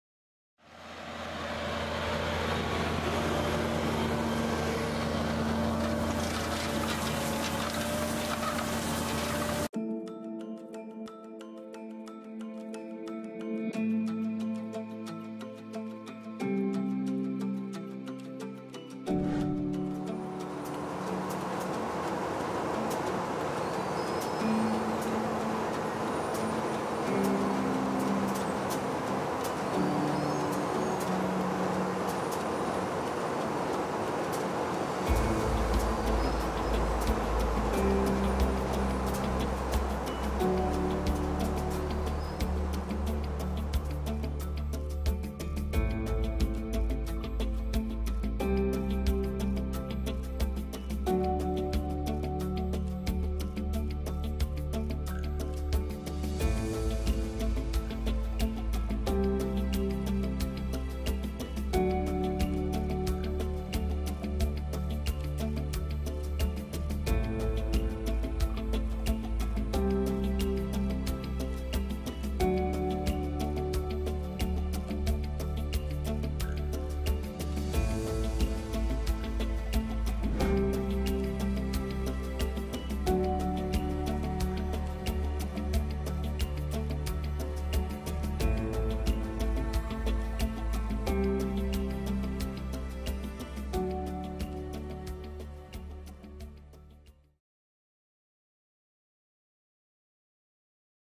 Pukekohe's onions
My time in an onion packhouse.